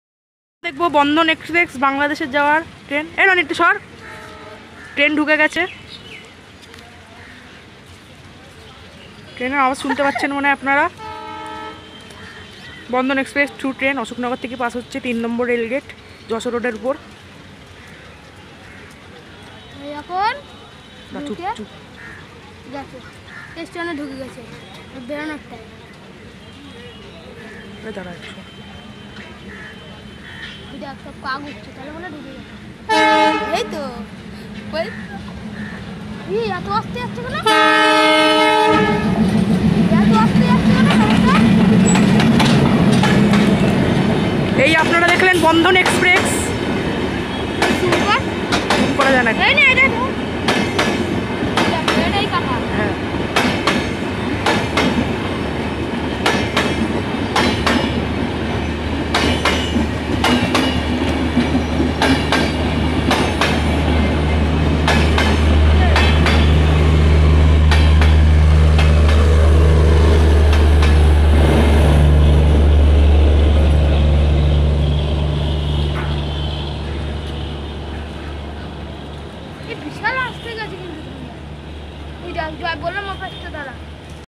Bandhan Express (Kolkata to Khulna) at Ashoknagar 3no Rail gate
Date -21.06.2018 at Ashoknagar by Pobitra Kaiputra